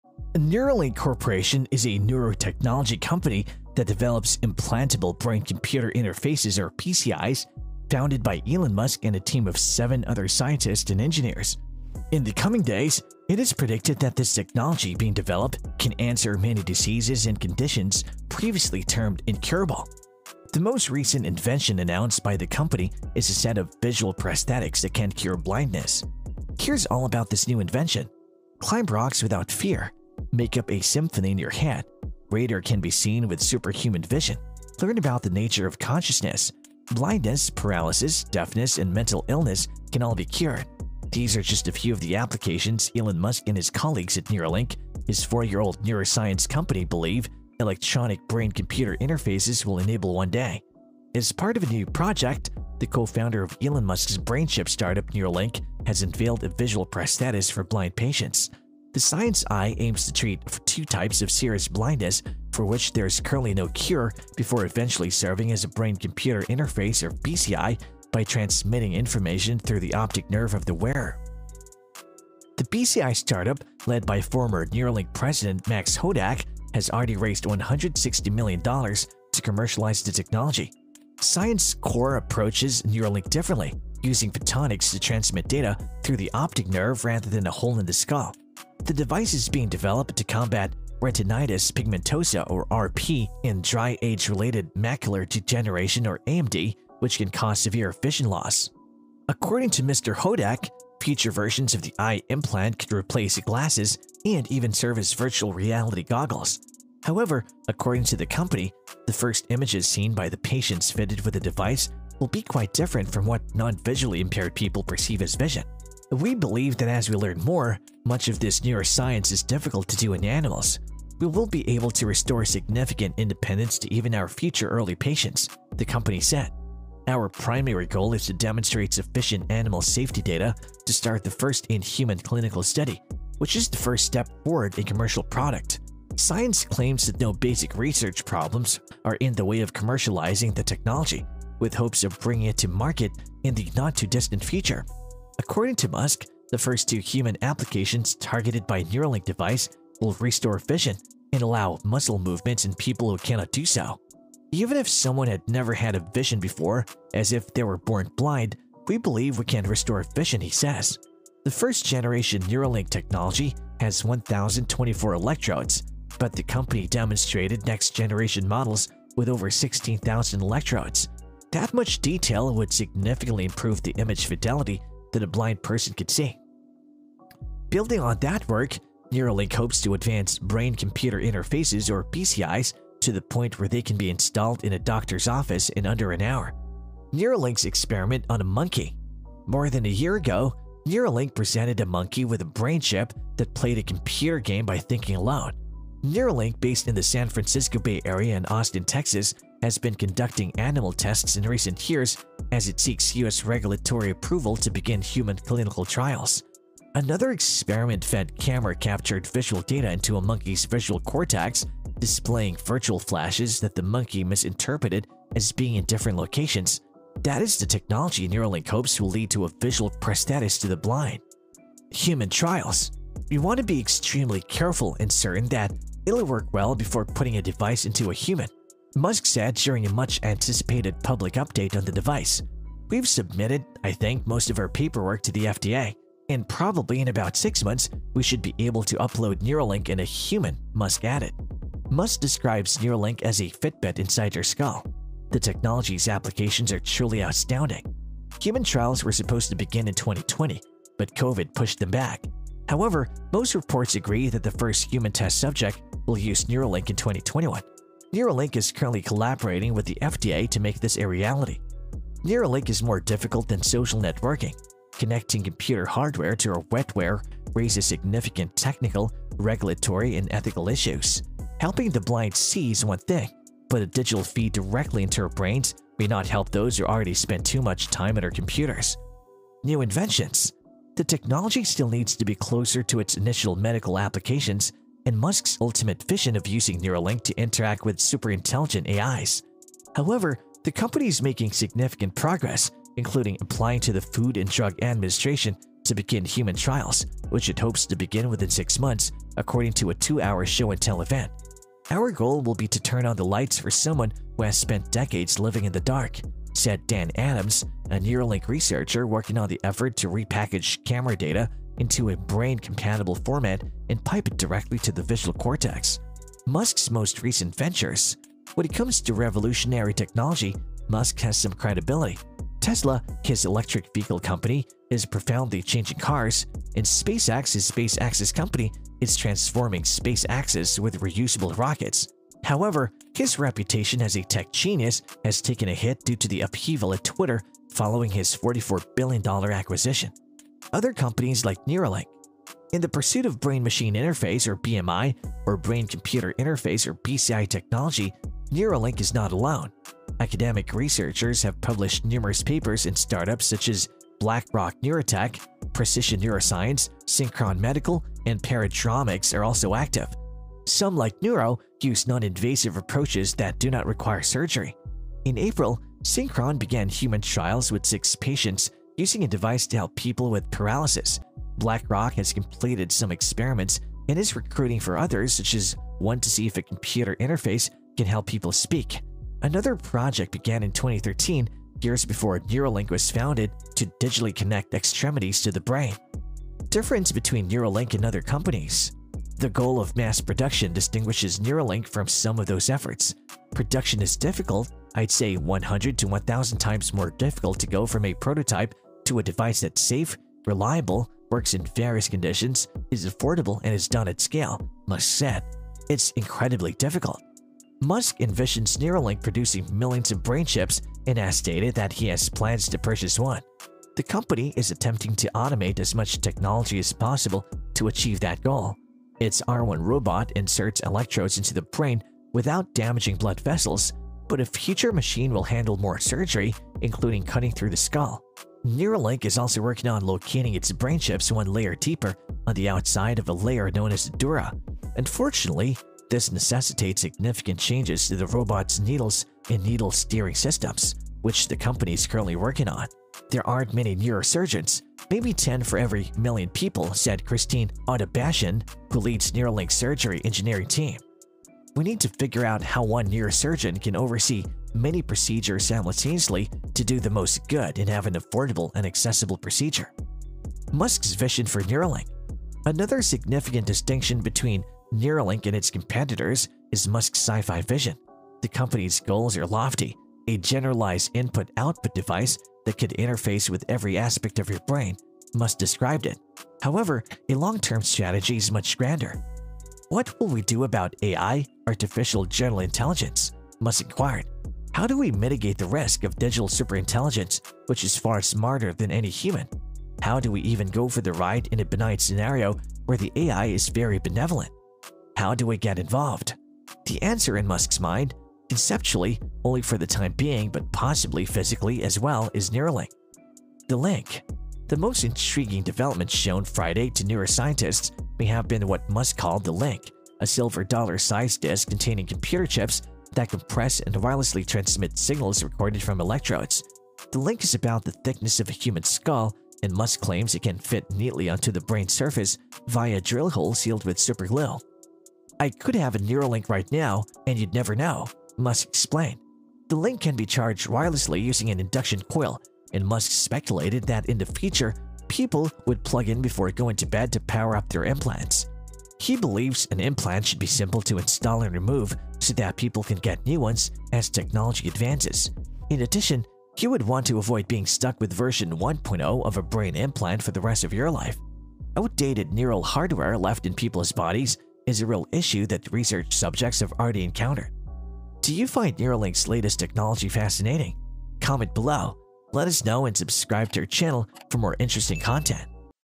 Major 2023 Update of Neuralink Will Cure Blindness!
Neuralink Corporation is a neurotechnology company that develops implantable Brain Computer interfaces (BCIs), founded by Elon Musk and a team of seven other scientists and engineers. In the coming days, it is predicted that this technology being developed can answer many diseases and conditions previously termed incurable. The most recent invention announced by the company is a set of visual prosthetics that can cure blindness!